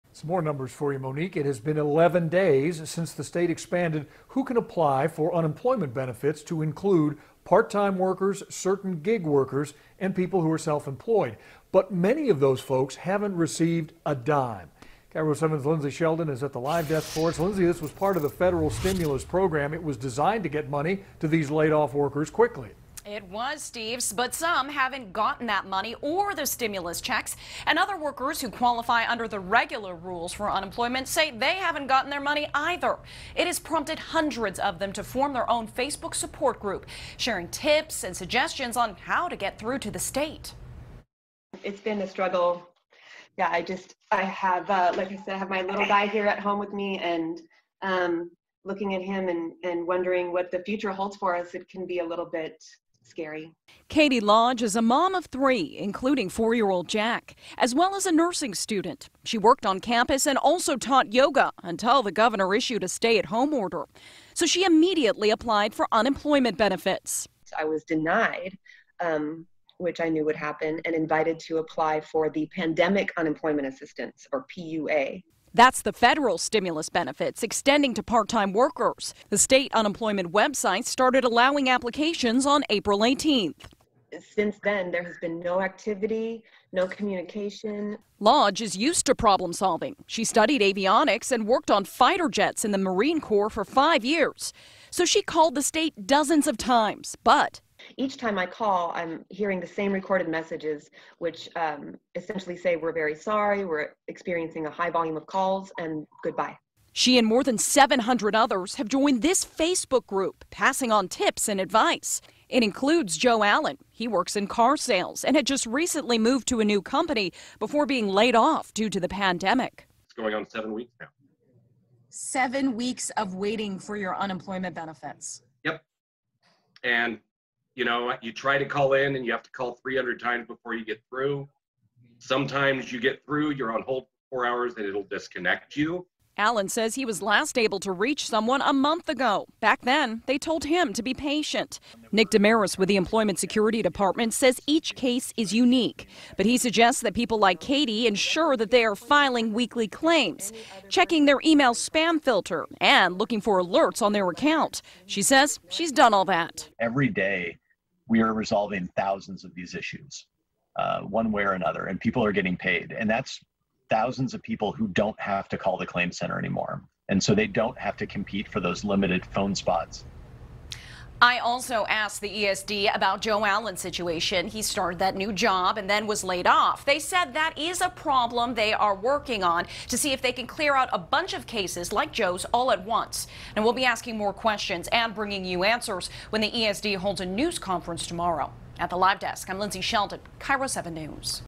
VIDEO: Hundreds waiting for unemployment benefits band together, share tips on getting state to resp
Hundreds waiting for unemployment benefits band together, share tips on getting state to respond